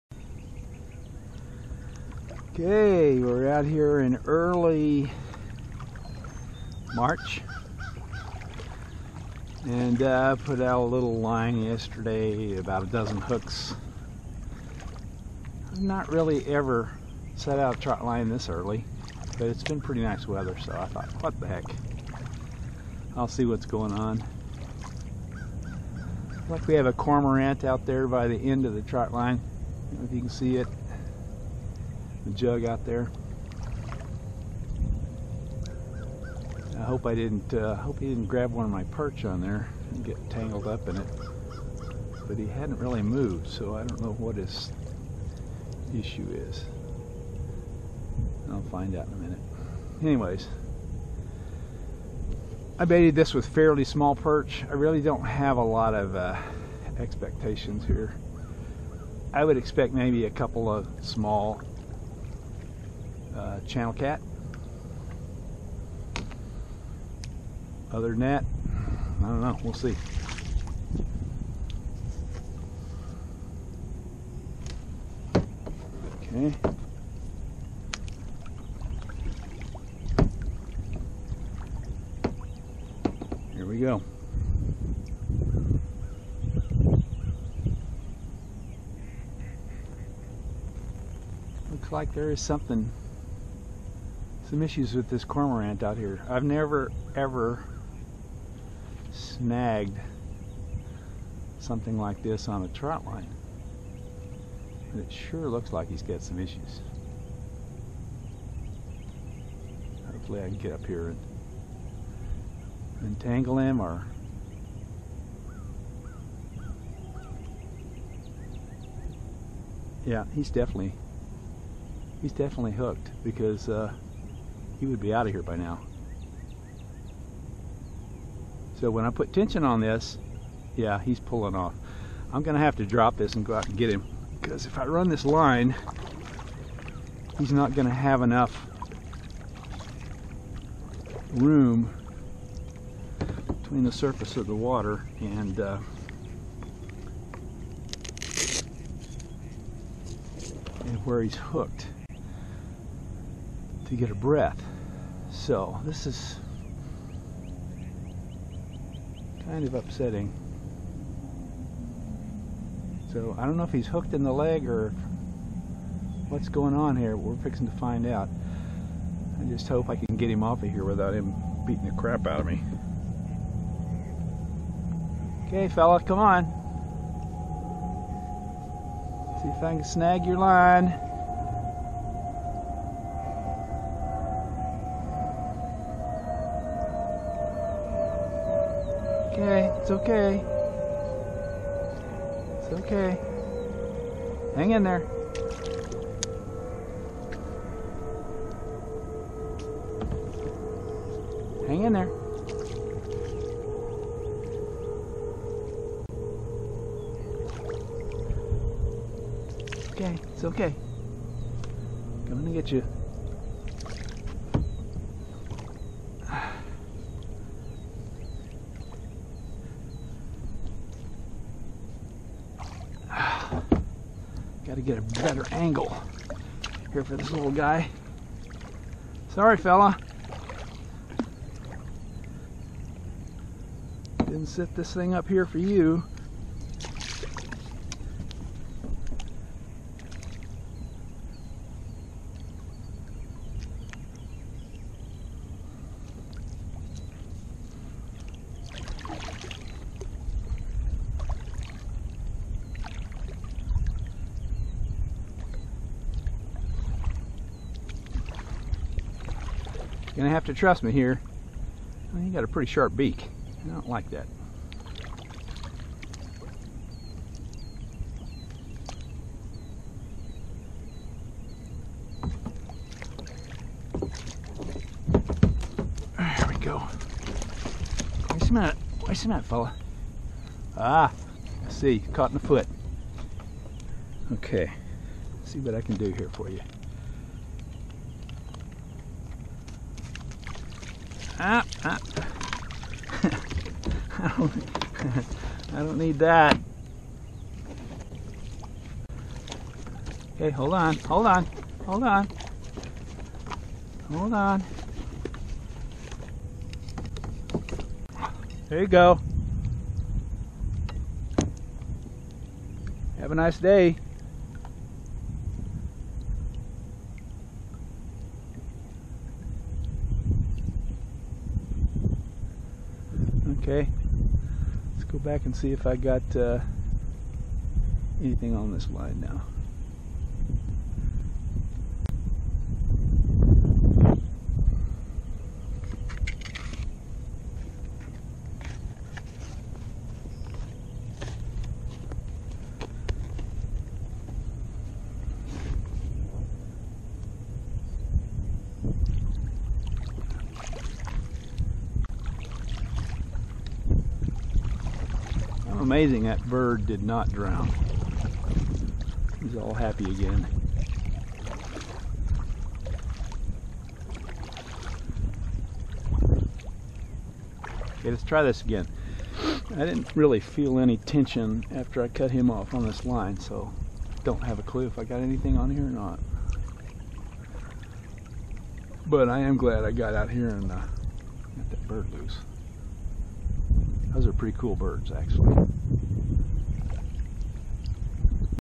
Cormorant encounter on Lewisville Lake
Cormorant gets snagged on a trotline on Lake Lewisville. Seems this guy was robbing my trotline and got caught in the foot. The line depth was about 10 feet under the surface. Never have seen this before.